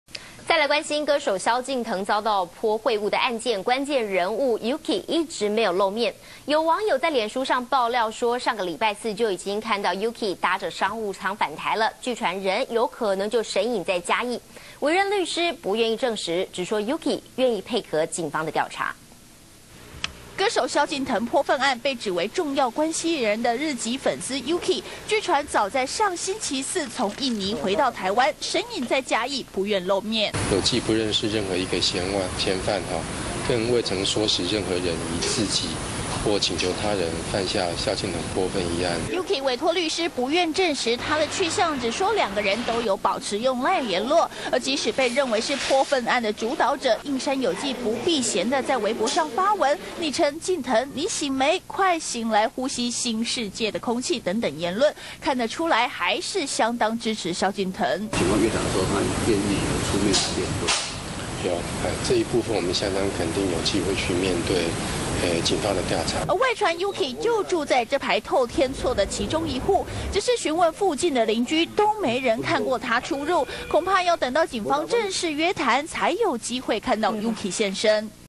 中視新聞》涉蕭敬騰潑穢案 Yuki嘉義老家神隱？
中視新聞》歌手蕭敬騰遭潑糞案，關鍵人物YUKI一直未曾露面，有網友在臉書爆料，上星期四就看到YUKI搭商務艙返台，據傳人有可能神隱在嘉義，委任律師不願證實，只說YUKI願意配合警方調查，也提醒任何在媒體上有關他談話，如有不實，將保留法律追訴權。